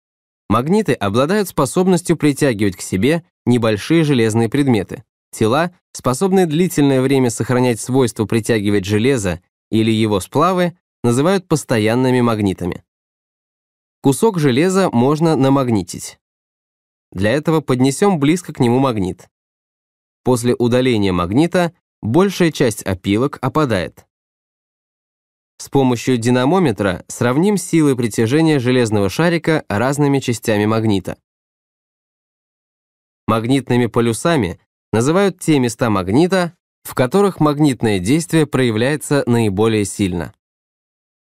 Опыты, доказывающие существование магнитного поля
Магниты обладают способностью притягивать к себе небольшие железные предметы.

Тела, способны длительное время сохранять свойство притягивать железо или его сплавы, называют постоянными магнитами.

Кусок железа можно намагнитить.
Для этого поднесем близко к нему магнит.
После удаления магнита, большая часть опилок опадает.
С помощью динамометра сравним силы притяжения железного шарика разными частями магнита.
Магнитными полюсами называют те места магнита, в которых магнитное действие проявляется наиболее сильно.